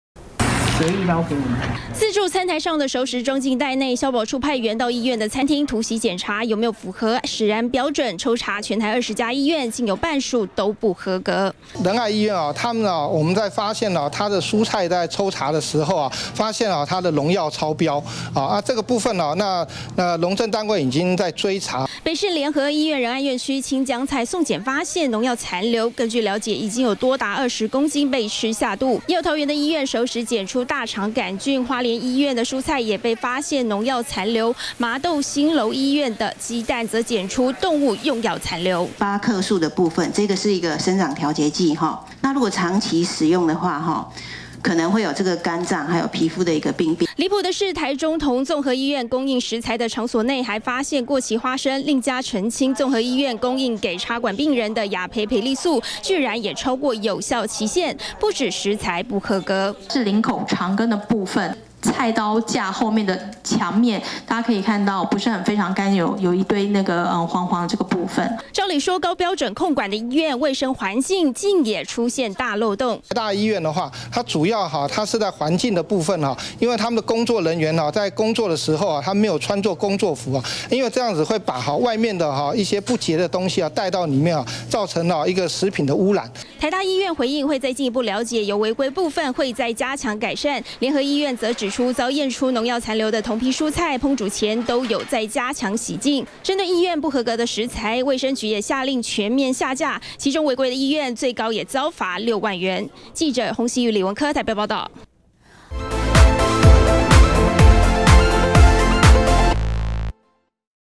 20161128中天新聞 醫院食安不合格！ 台大、北市聯醫上榜
醫院膳食竟也出現食安問題！行政院消保處，針對20家醫院，抽查發現有多達10家不合格，包括北市聯合醫院仁愛院區的蔬菜有農藥殘留，台大醫院供食的場所，員工沒有穿著工作服，甚至台中澄清綜合醫院還出現過期食物。